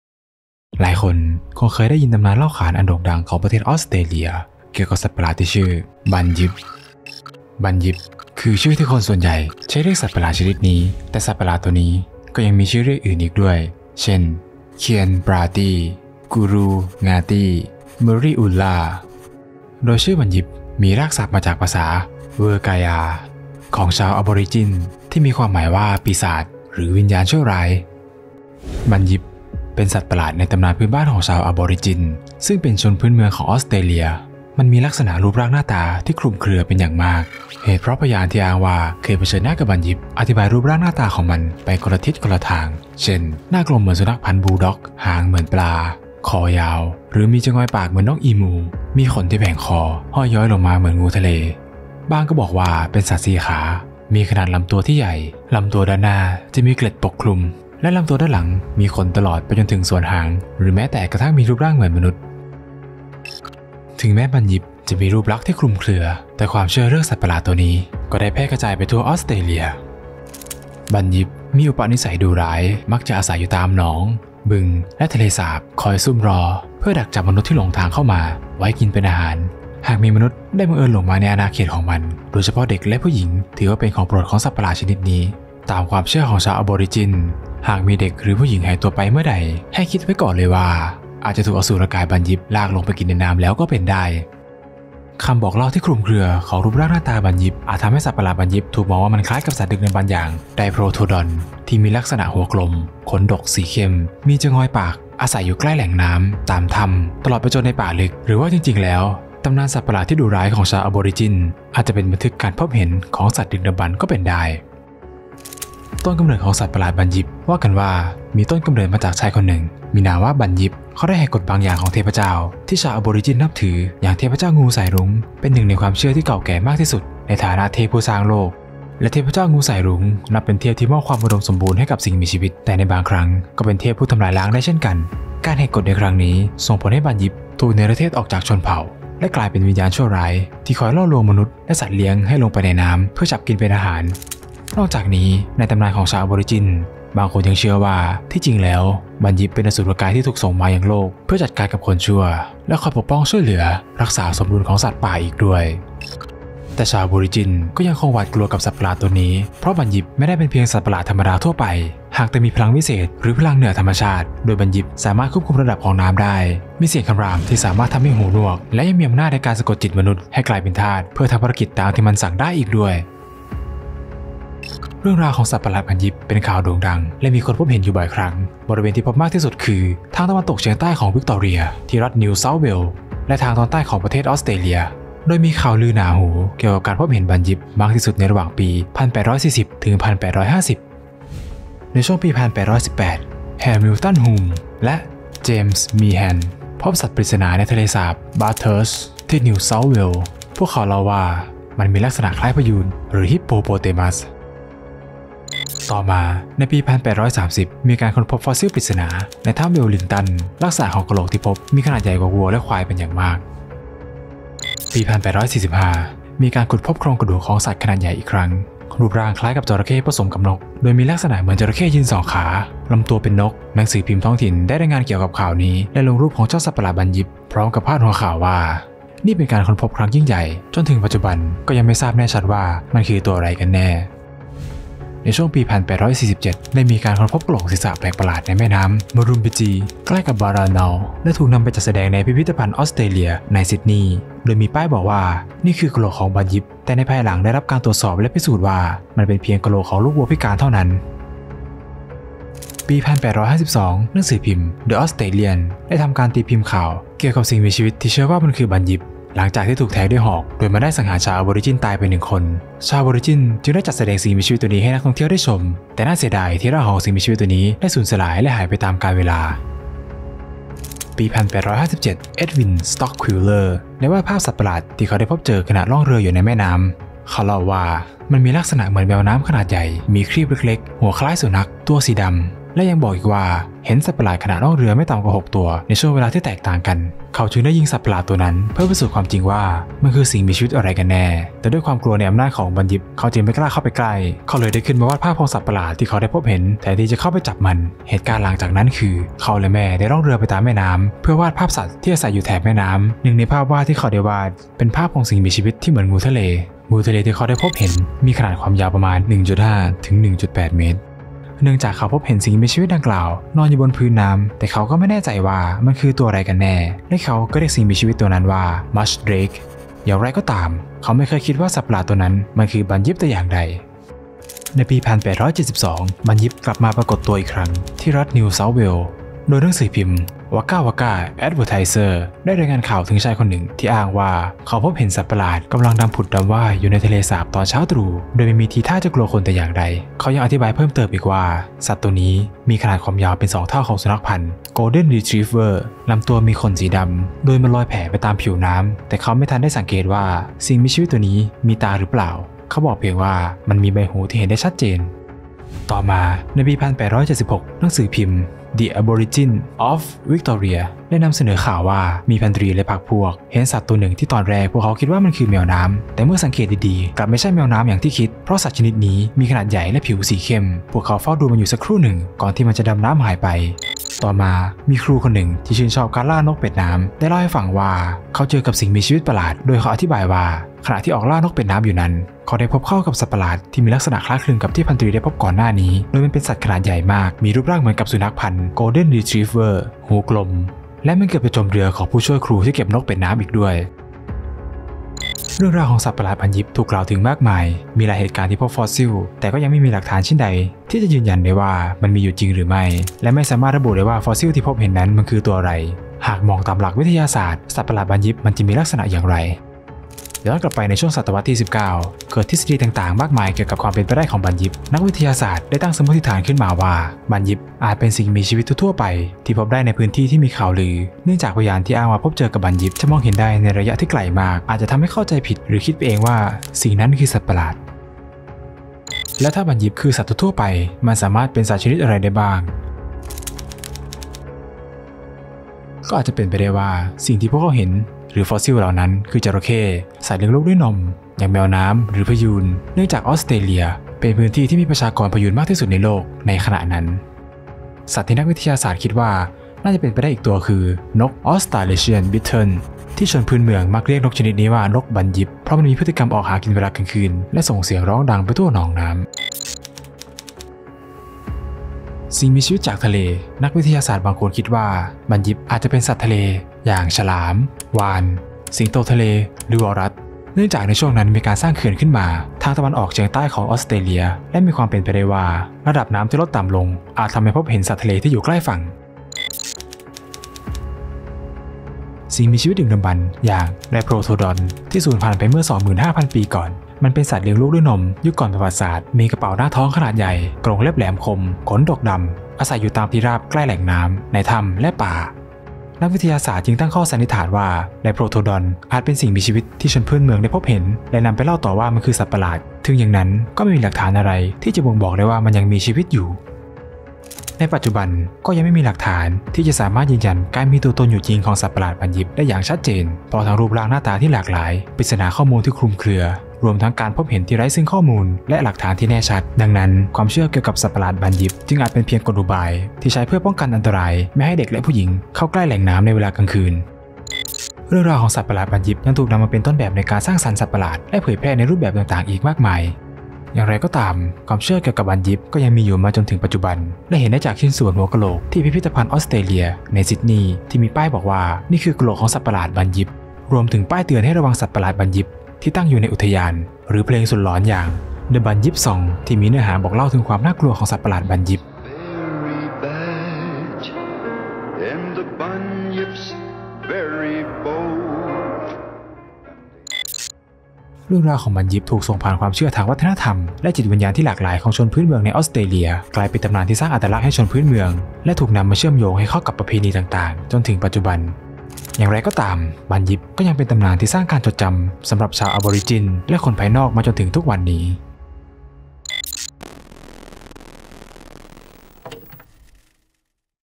Bunyip: อสูรกายกินคน ตำนานที่ไม่เคยเลือนหายไปจากลุ่มน้ำออสเตรเลีย
Bunyip เป็นสัตว์ประหลาดในตำนานพื้นบ้านของชาว Aborigins ในประเทศออสเตรเลีย
Bunyip มีลักษณะรูปร่างหน้าตาที่คลุมเครือมาก เพราะพยานที่อ้างว่า.. เคยเผชิญหน้ากับ Bunyip อธิบายรูปร่างหน้าตาของมันไปคนละทิศทาง
แล้วหน้าตาของมันจะออกมาเป็นแบบไหน ตามไปเหลากันเลยครับ

รวบรวม และเรียบเรียงเนื้อหาจาก: